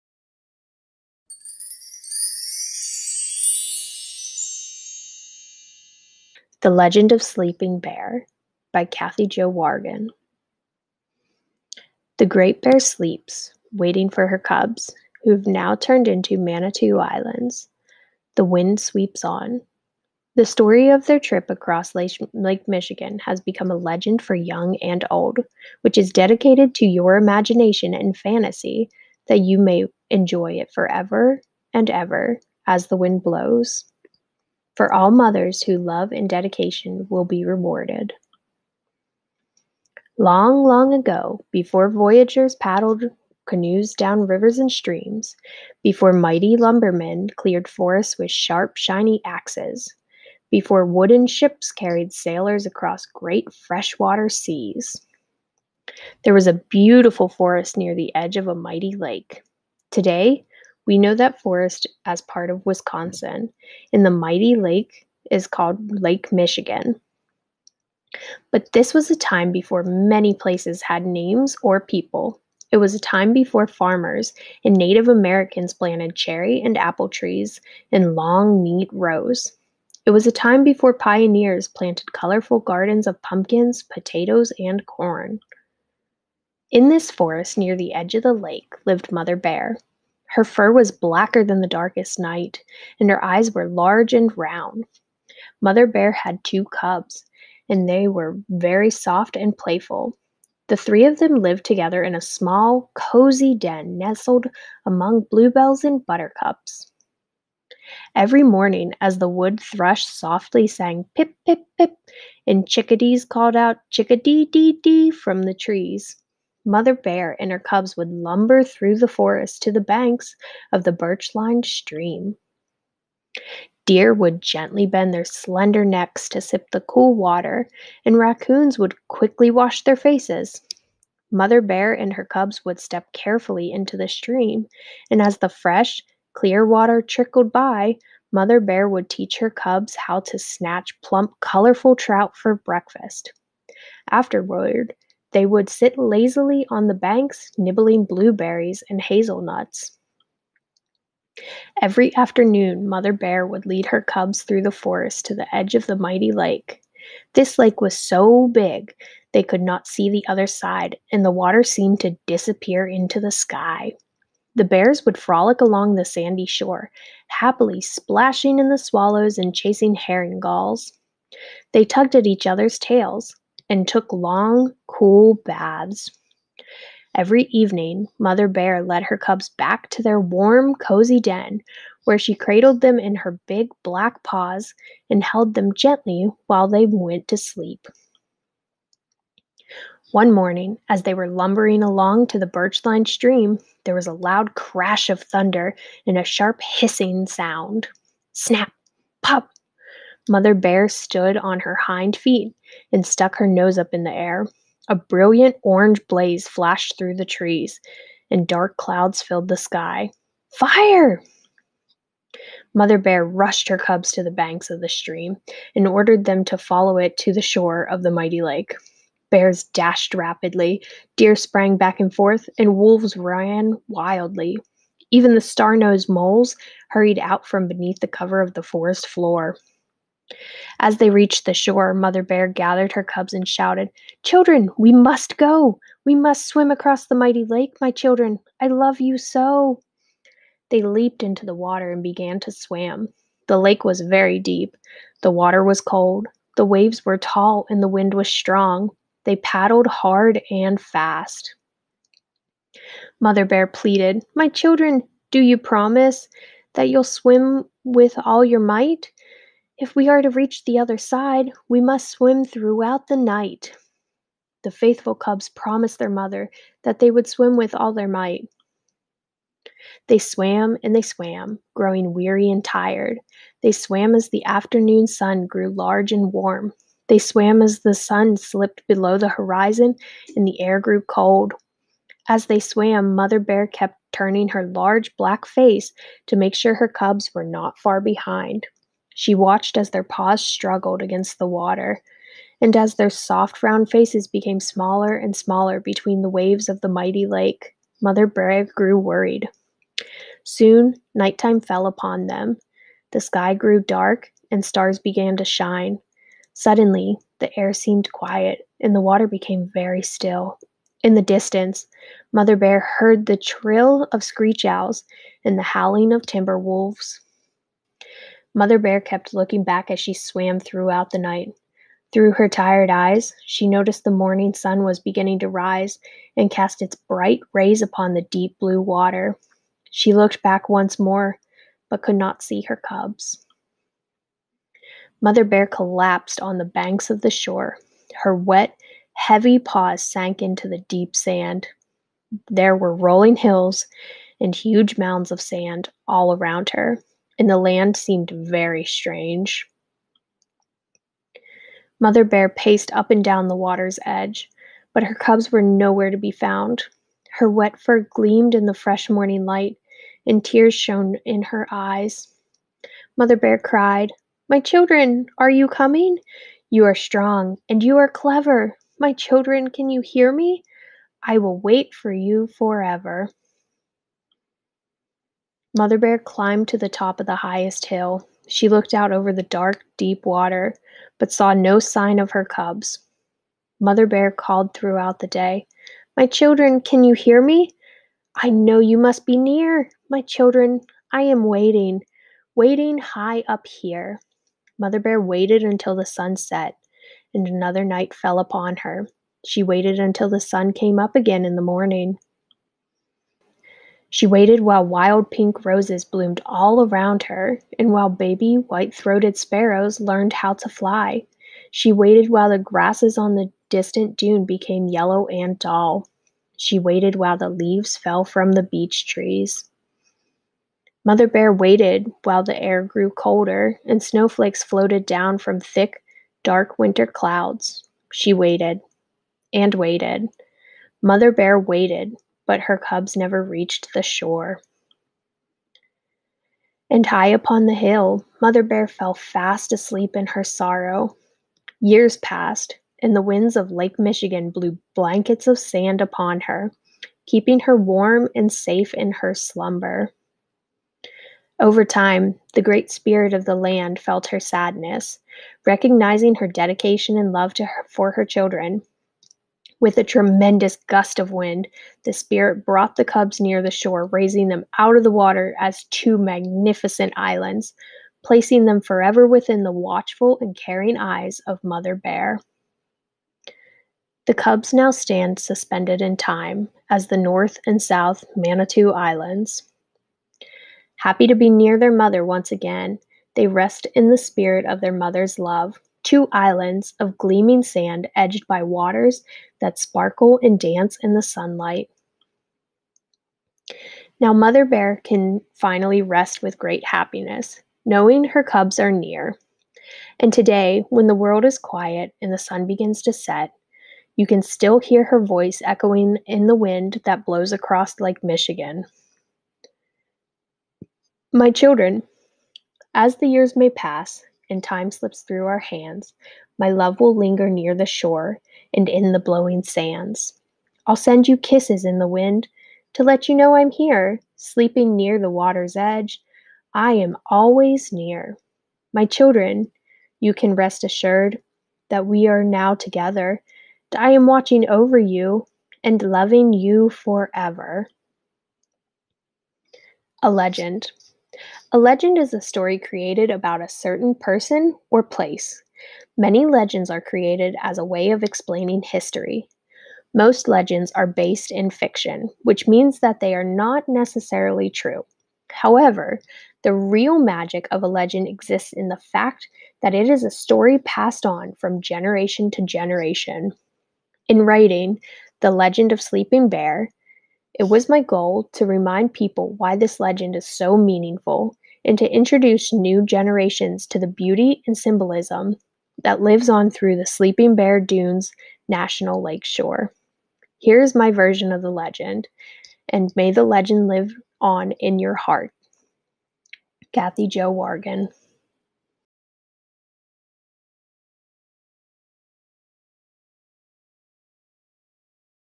The Legend of Sleeping Bear | Literacy Lab Read Aloud
Ms. Gretta reads The Legend of Sleeping Bear by Kathy-jo Wargin and illustrated by Gijsbert van Frankenhuyzen.